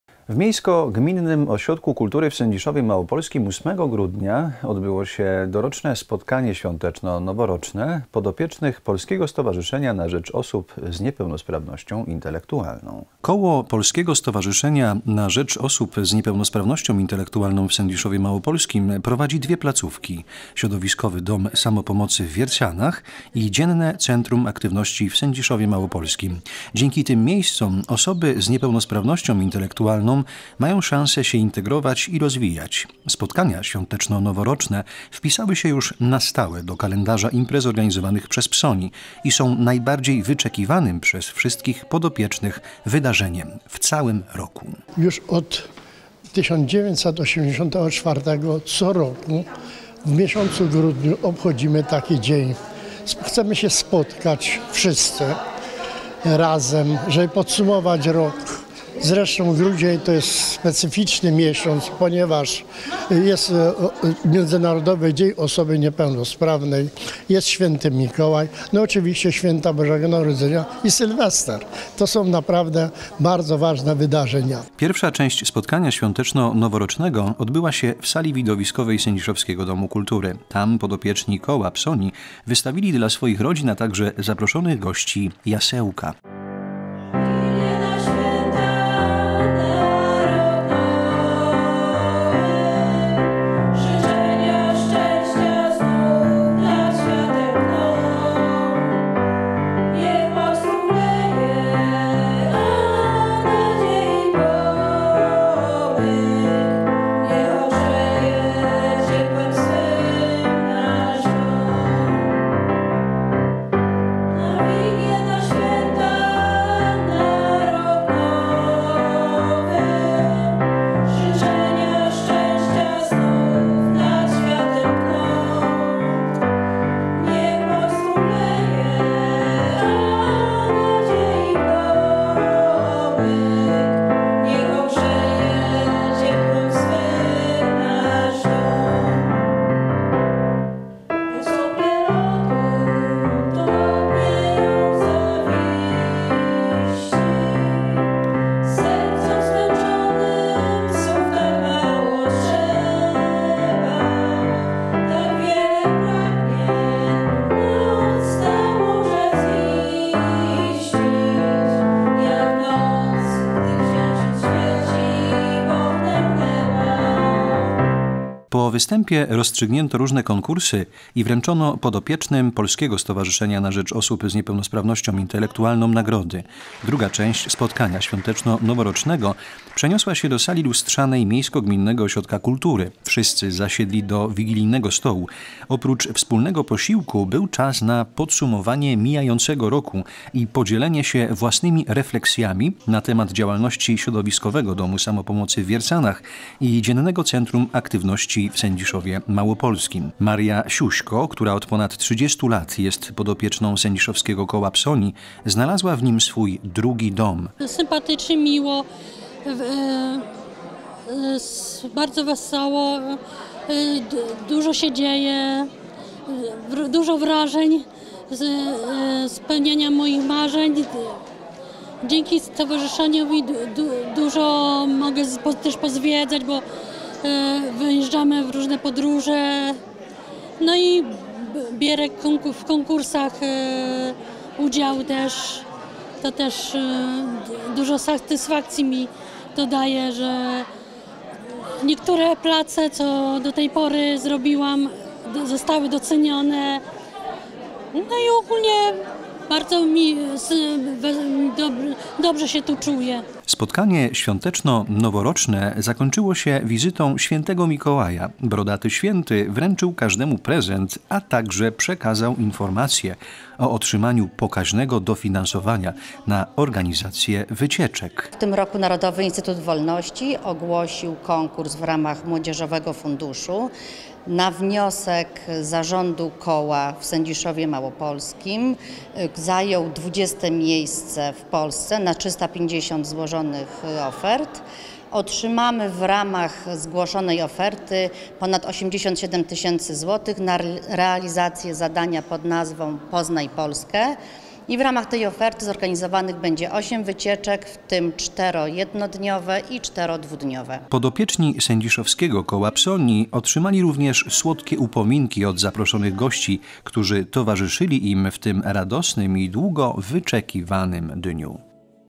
Sędziszowskie koło PSONI podsumowało rok
W Miejsko-Gminnym Ośrodku Kultury w Sędziszowie Małopolskim 8 grudnia odbyło się doroczne spotkanie świąteczno-noworoczne podopiecznych Polskiego Stowarzyszenia na Rzecz Osób z Niepełnosprawnością Intelektualną.